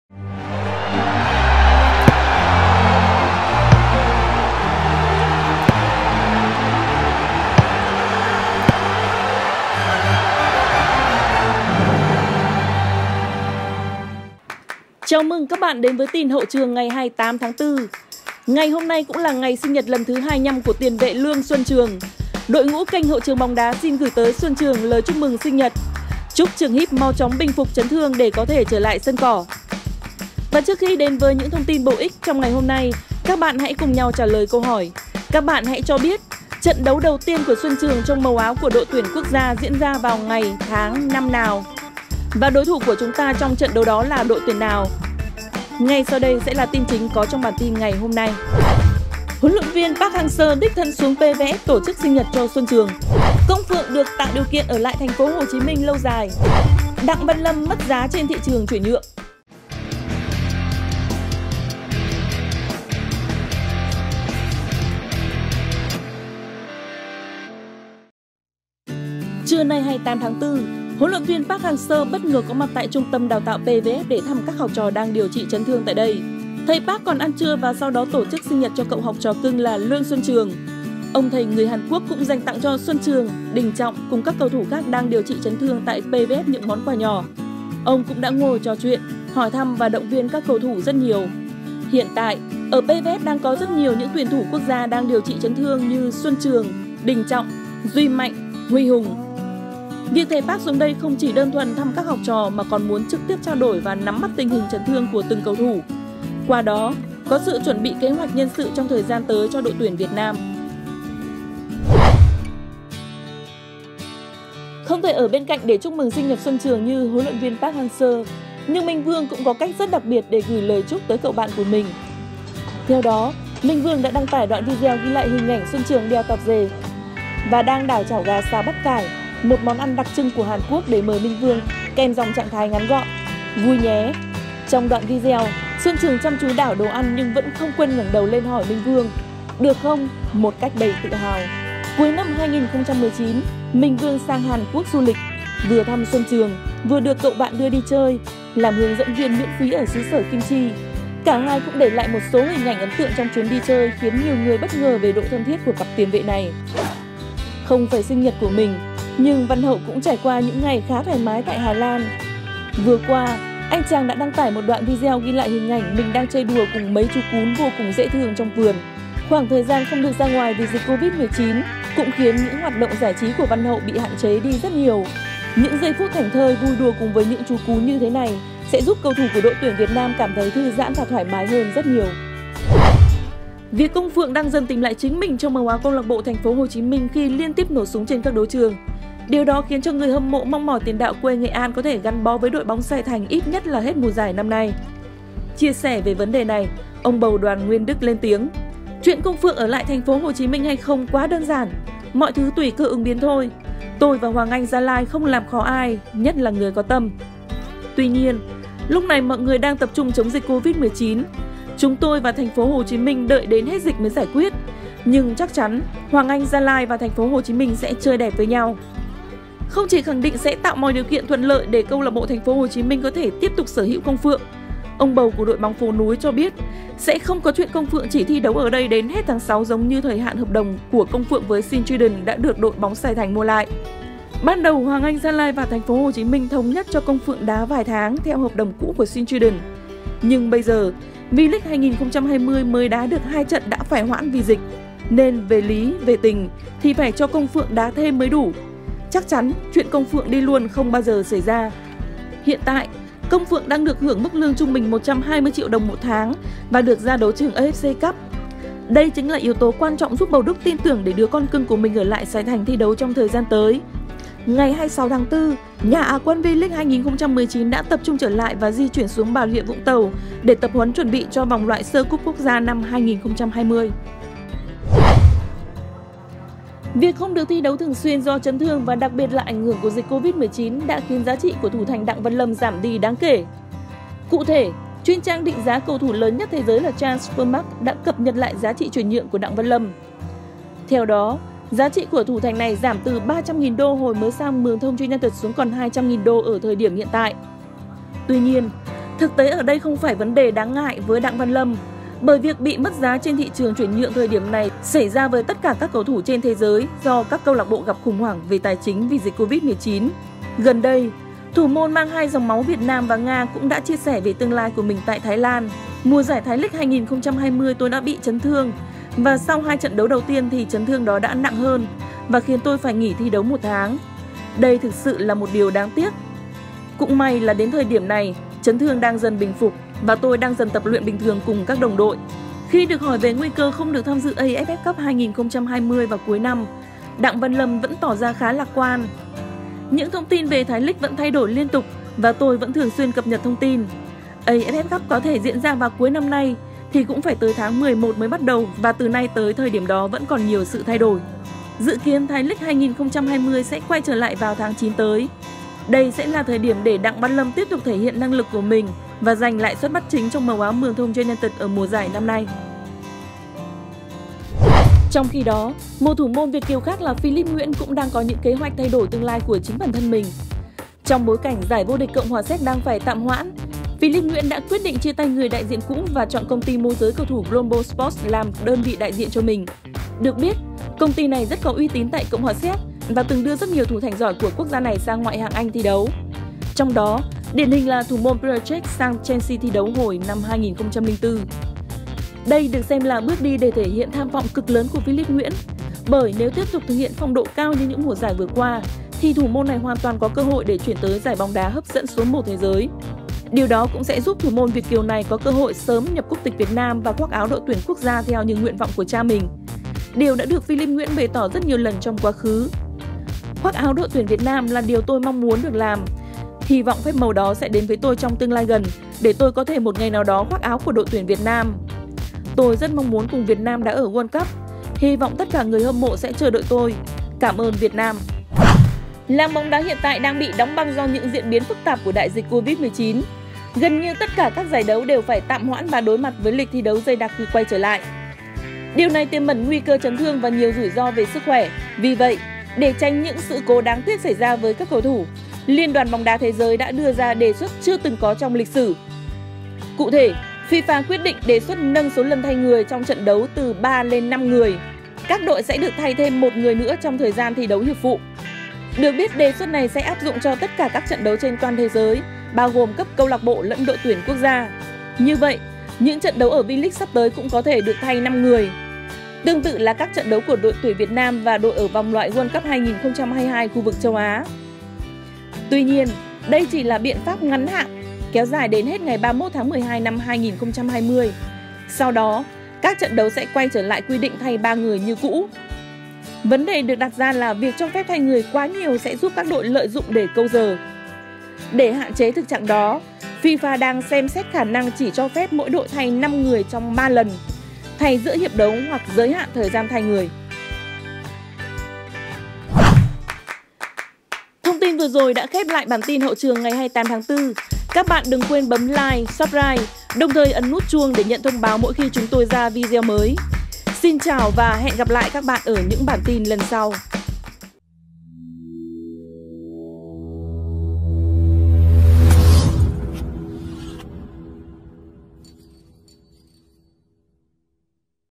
BÓNG ĐÁ VIỆT NAM 28/4 I THẦY PARK TỔ CHỨC SINH NHẬT CHO XUÂN TRƯỜNG, VĂN LÂM BỊ MẤT GIÁ
ĐIỂM TIN:
- HLV PARK HANG-SEO ĐÍCH THÂN XUỐNG PVF TỔ CHỨC SINH NHẬT CHO XUÂN TRƯỜNG
- CÔNG PHƯỢNG ĐƯỢC TẠO ĐIỀU KIỆN Ở LẠI THÀNH PHỐ HỒ CHÍ MINH LÂU DÀI
- ĐẶNG VĂN LÂM MẤT GIÁ TRÊN THỊ TRƯỜNG CHUYỂN NHƯỢNG
✔Một sản phẩm của TT Football
©Bản quyền video thuộc về kênh Youtube Hậu Trường Bóng Đá. Đề nghị không reup dưới mọi hình thức.